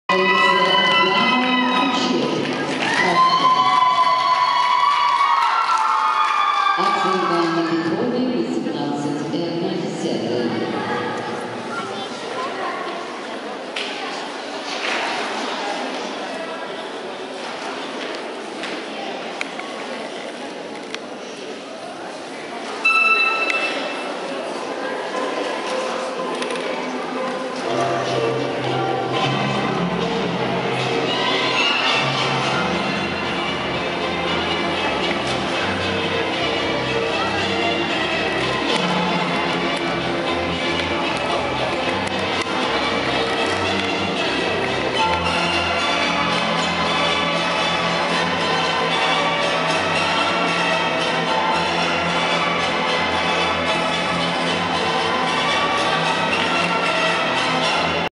Кучер Яна 2001 г.р. б/п г.Шахты
Первенство Профсоюзов по художественной гимнастике 23.09.11 г.Люберцы